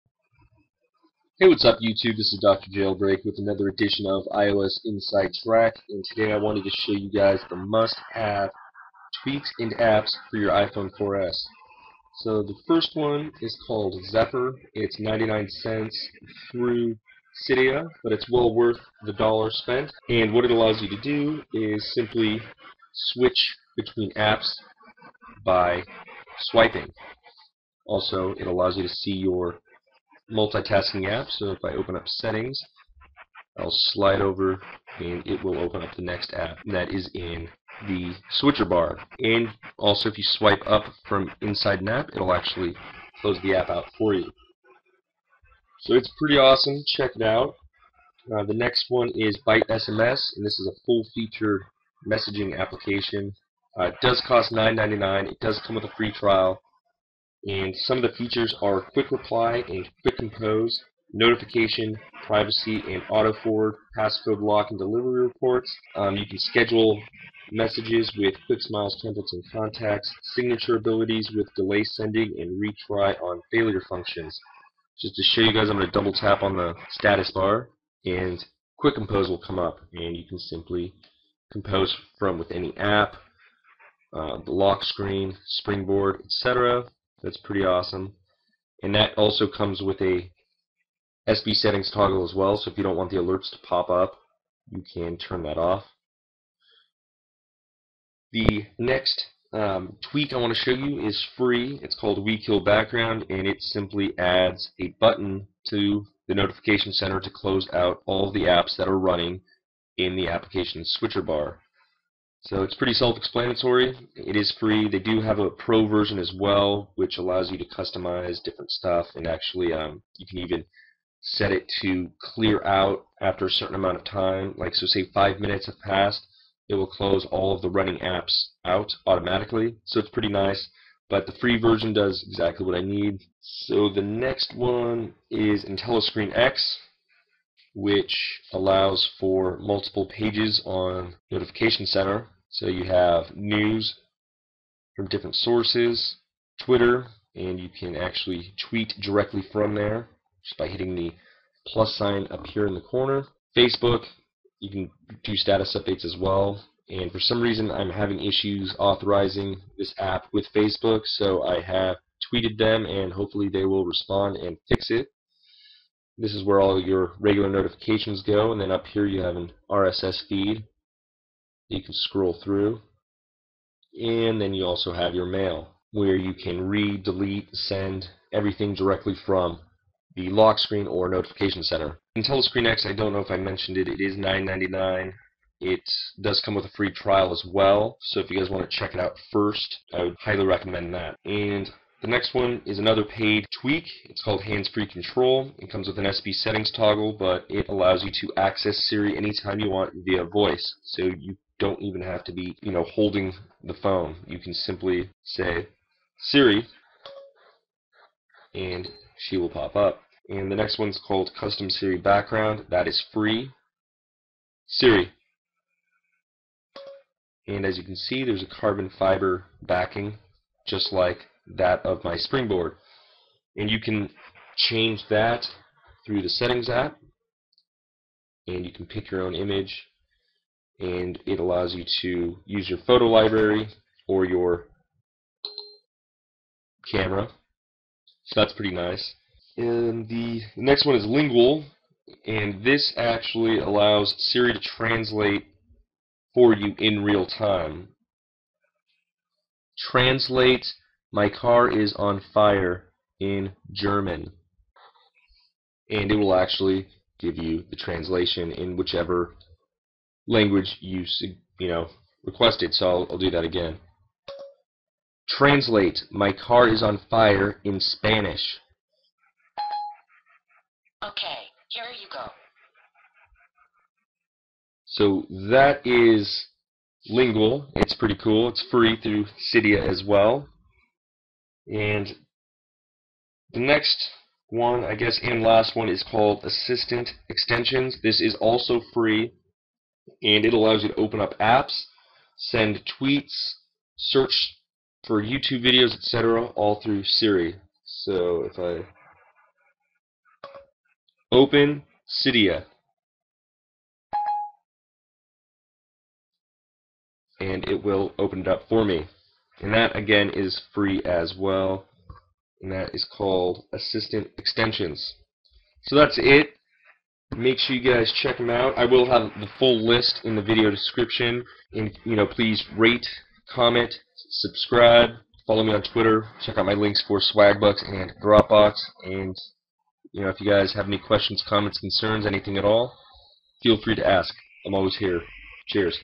iOS INSIDE TRACK: MUST HAVE APPS AND TWEAKS FOR iPHONE 4S!
This is a list of must have tweaks and apps for your iPhone 4S

Zephyr: $0.99
WeeKillBackground: Free
Hands-Free Control: $2.99
Custom Siri Background: Free
Lingual: Free
AssistantExtensions: Free